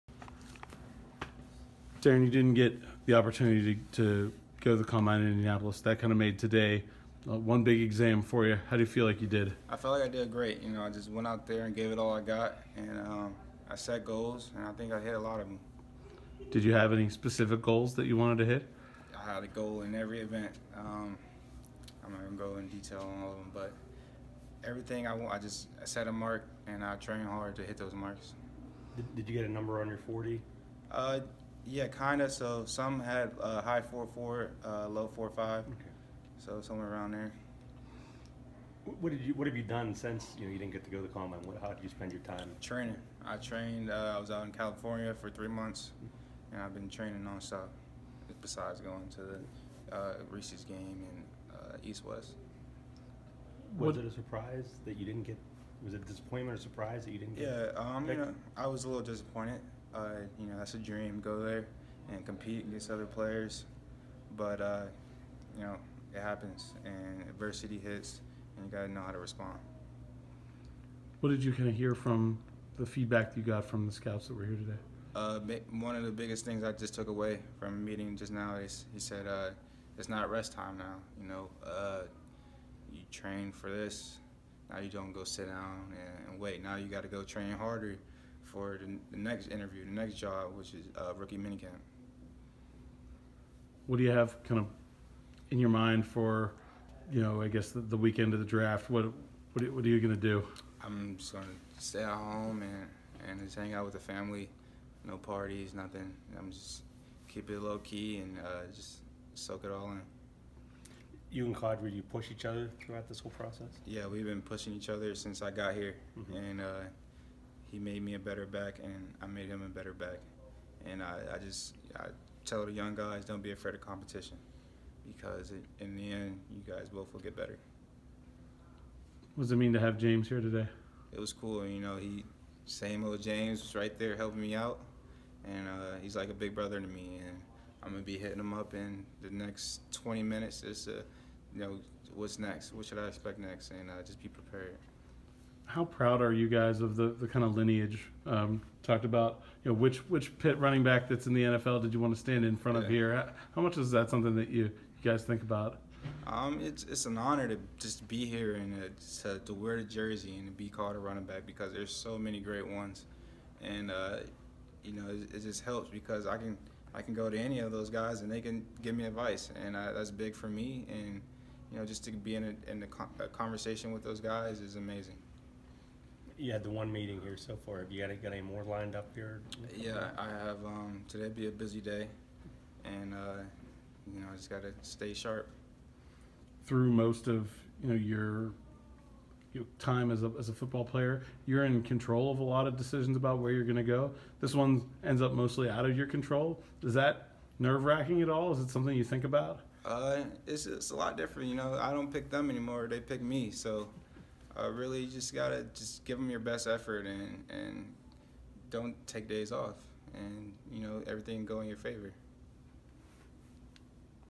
Pitt RB Darrin Hall 3/20/19 | Pitt Pro Day | PSN
Pitt running back Darrin Hall speaks with the media, including Alan Saunders of Pittsburgh Sports Now, after Pitt's pro day on Wednesday, March 20, 2019.